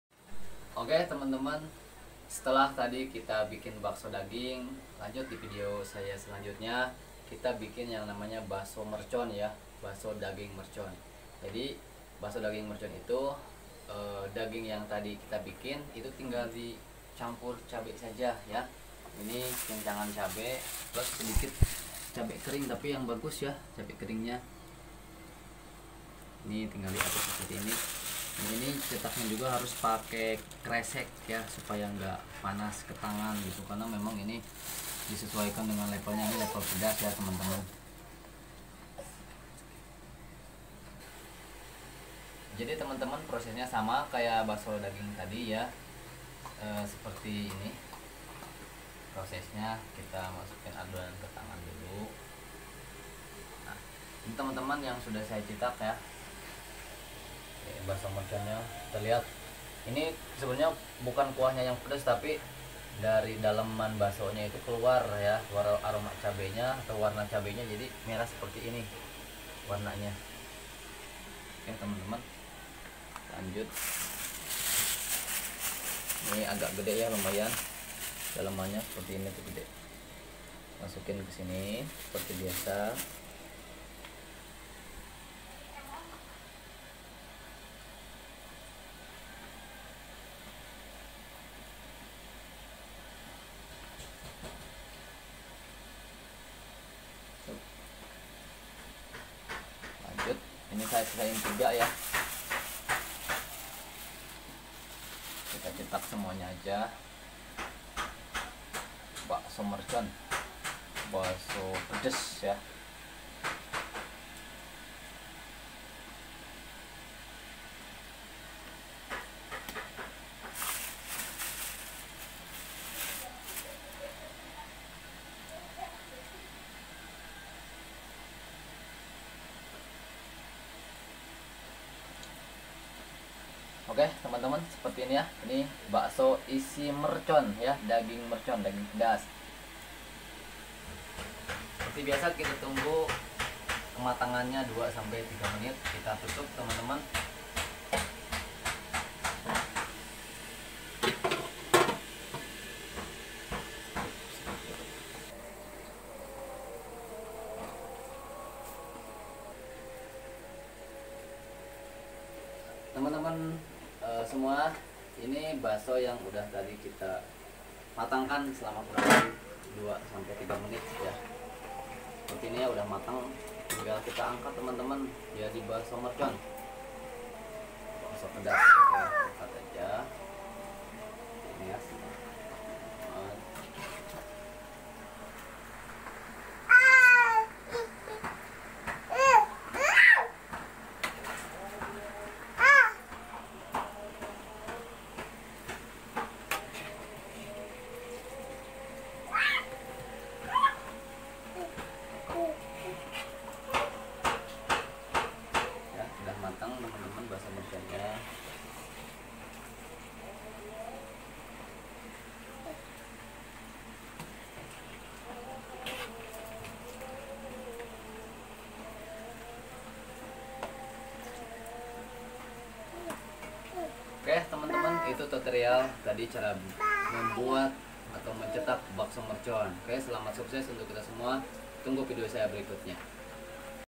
Cara mudah membuat bakso mercon,viral,pedes nampol
cara membuat bakso mercon
Sama srperti halnya membuat bakso isi daging cuma si daging di kasih cabai halus nya...